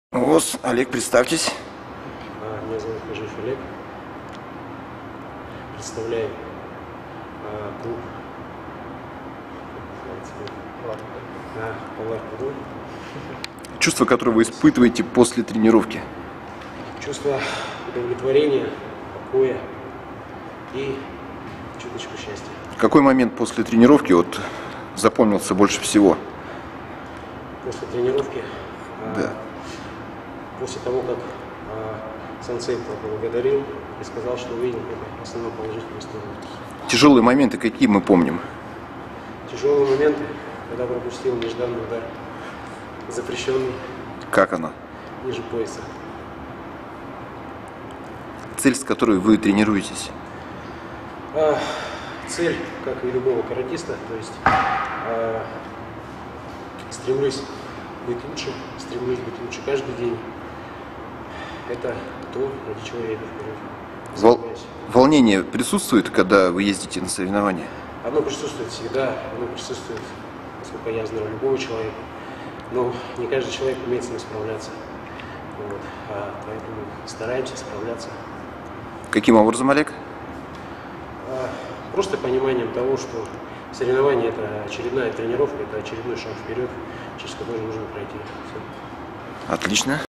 Соревнования для меня это очередной шаг вперёд через который нужно пройти. Хажиев Олег - Мурманск
Хажиев Олег - Соревнования для меня это очередная тренировка, очередной шаг вперёд, через который нужно пройти